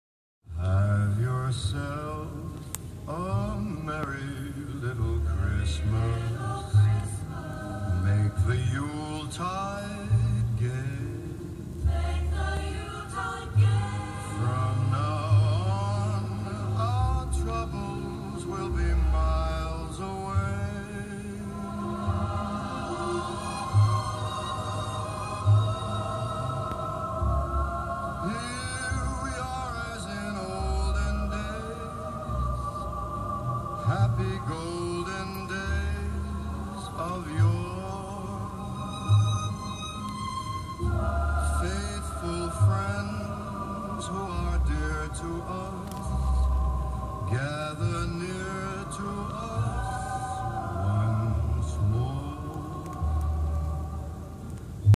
Frank Sinatra and Douglas Fir. Lights and Music.
Sequenced Lights and radio free Sinatra.